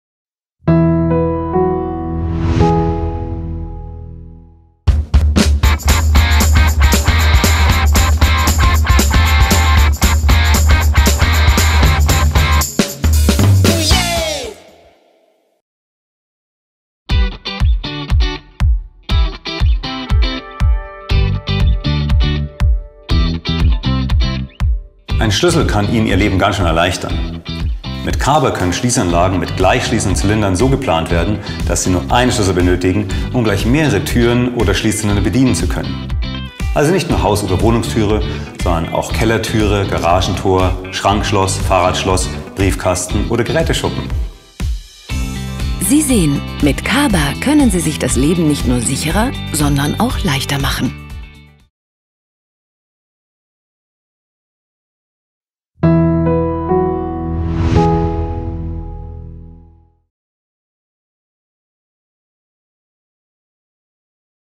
Mechanische Schliesssysteme von dormakaba – Ihr Schlüssel für alles
Mit den mechanischen Schliesssystemen von dormakaba, ist es möglich, einen Schlüssel so zu produzieren, dass er mit allen eingesetzten Zylindern übereinstimmt. So können Sie jederzeit, alle Ihre Türen mit nur einem Schlüssel öffnen. Einfacher, sicherer und besser geht’s nicht…